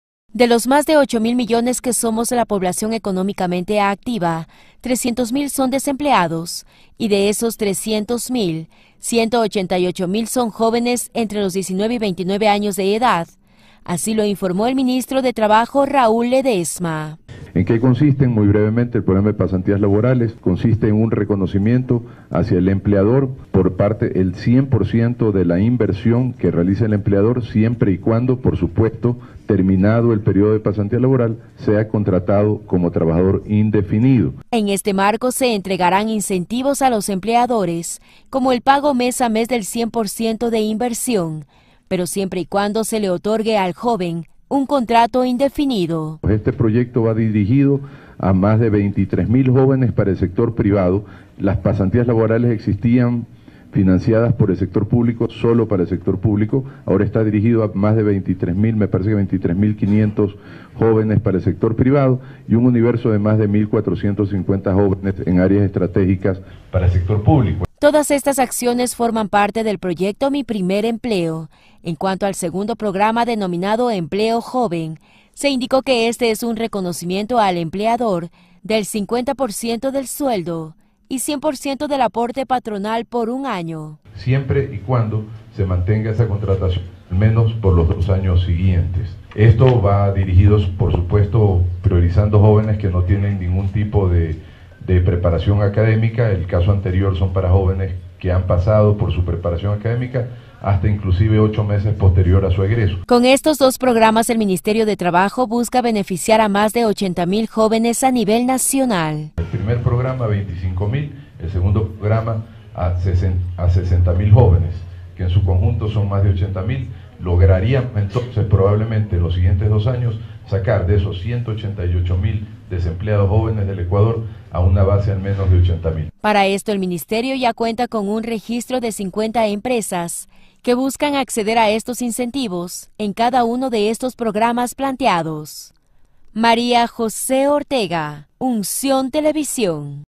Ministerio de Trabajo busca abrir 80 mil plazas de trabajo para jóvenes
Crear más de 80 mil plazas de empleo para los jóvenes es el objetivo que tiene el Ministro de Trabajo Raúl Ledesma, por lo que busca poner en marcha los programas denominados mi primer empleo y  empleo joven, los mismos que otorgan incentivos a las empresas que acojan estas iniciativas.